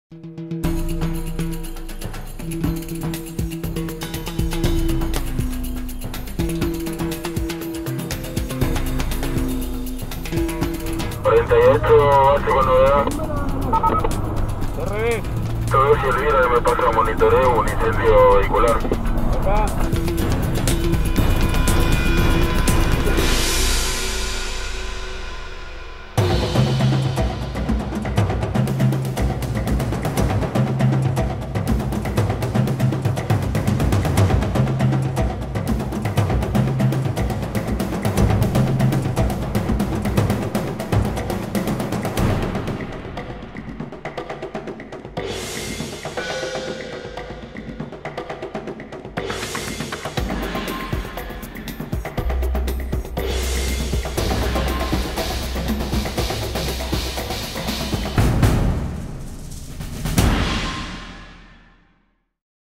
Gracias a las cámaras de Tigre, el Sistema de Protección Ciudadana controló un peligroso incendio
Gracias a las cámaras de Tigre, el Sistema de Protección Ciudadana controló un peligroso incendio vehicular